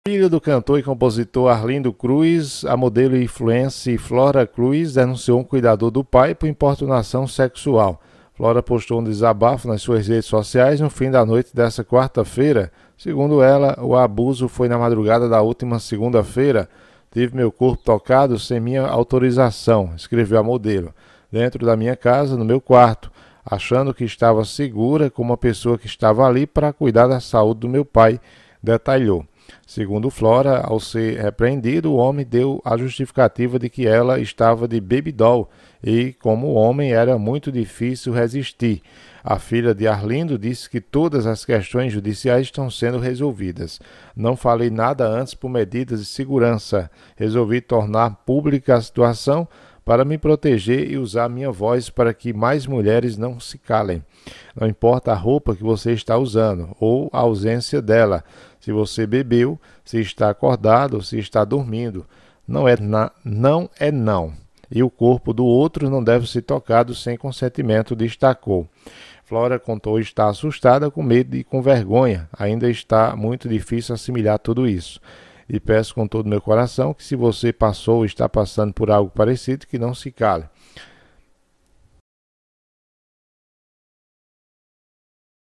Filha de Arlindo Cruz denuncia cuidador do pai por importunação sexual
Por g1 Rio
27/04/2023